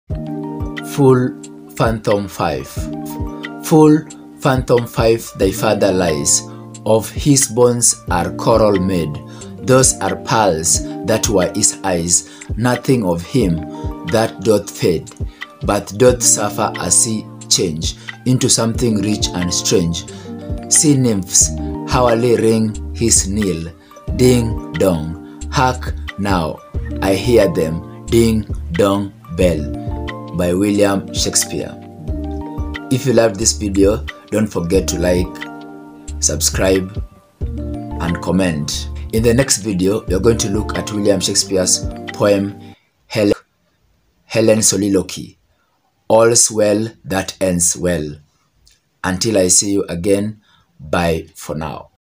english Full Fathom Five by William Shakespeare
Full fathom five thy father lies;
Of his bones are coral made;
Those are pearls that were his eyes:
Nothing of him that doth fade
But doth suffer a sea-change
Into something rich and strange.
Sea-nymphs hourly ring his knell:
Ding-dong.
Hark! now I hear them,--ding-dong, bell.